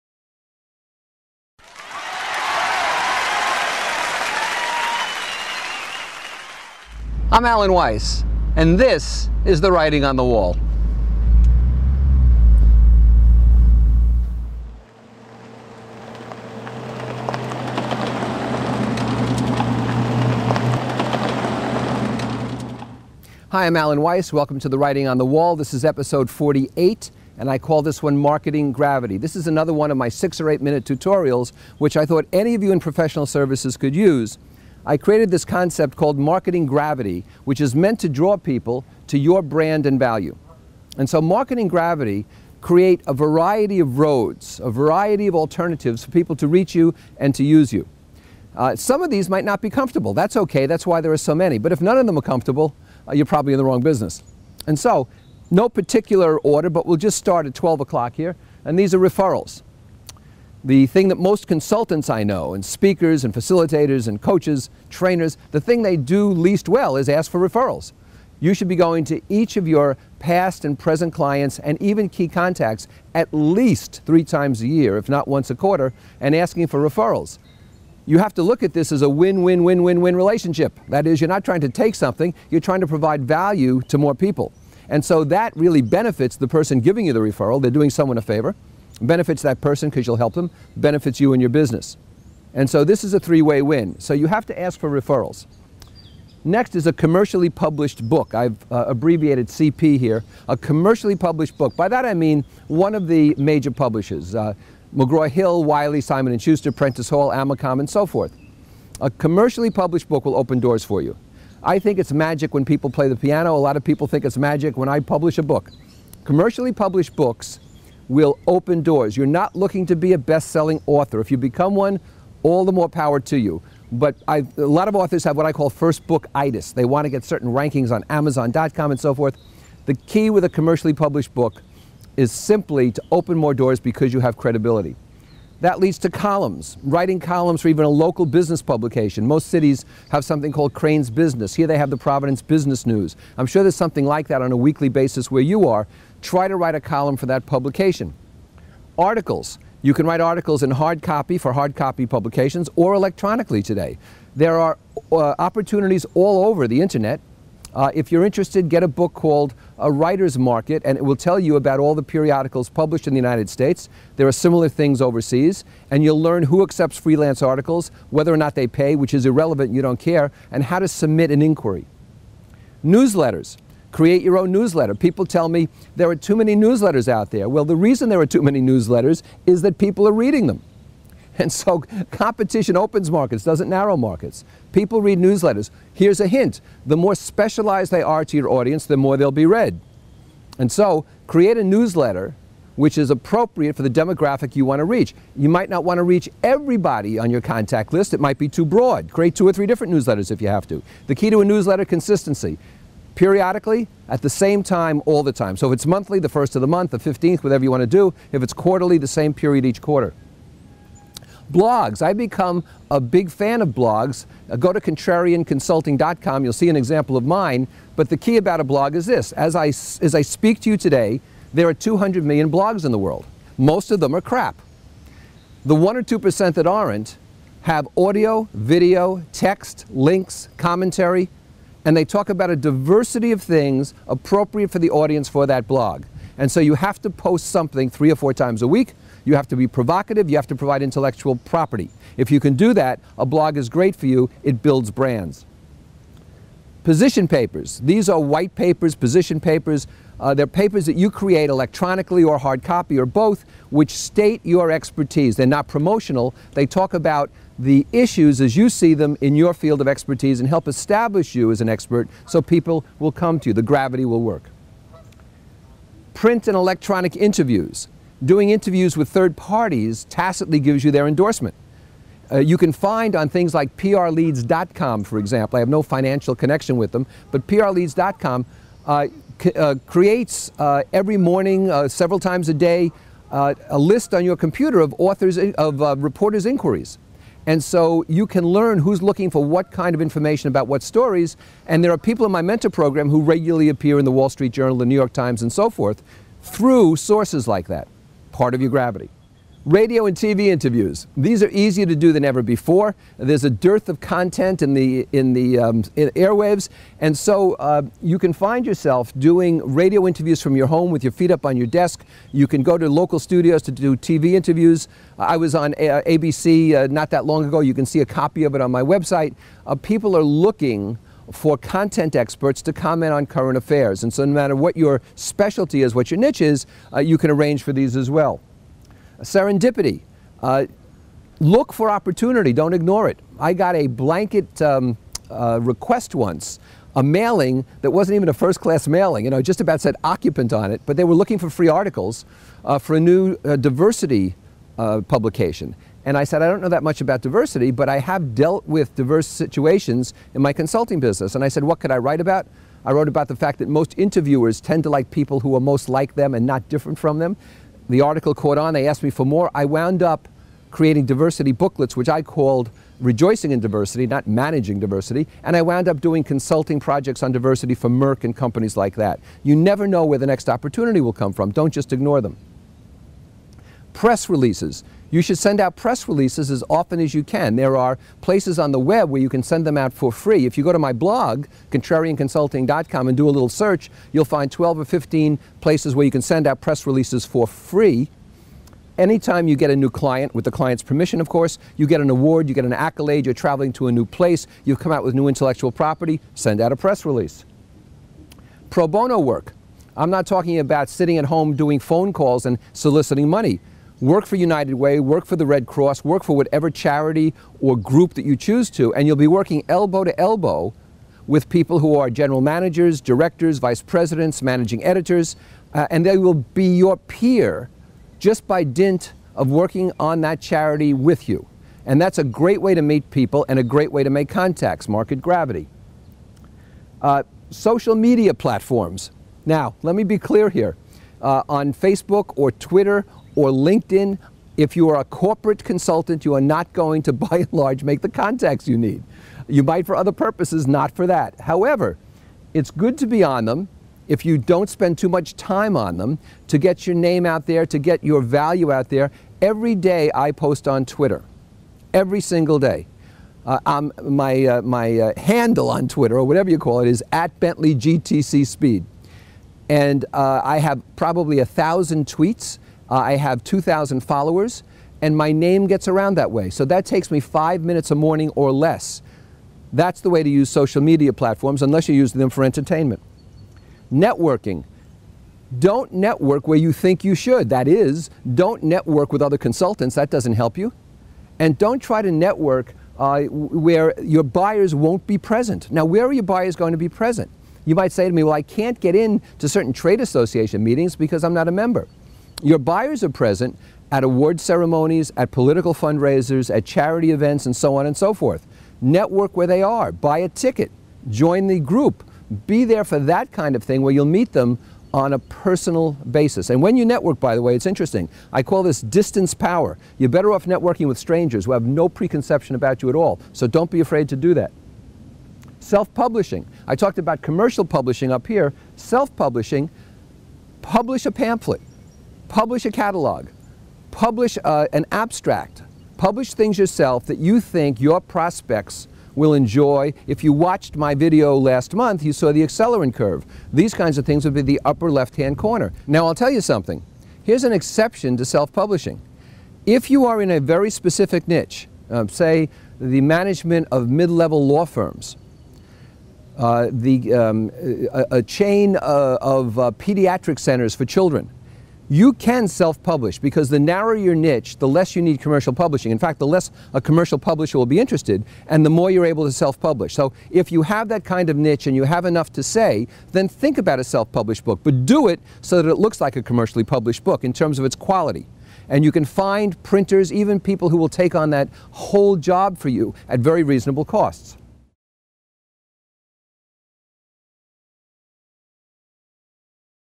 Marketing Gravity (Episode 48 Part 1 of 2)
A monthly video series hosted by Alan Weiss and featuring Koufax the Wonder Dog, addressing topics of global import, professional importance, or whatever has distracted or irritated Alan at the moment.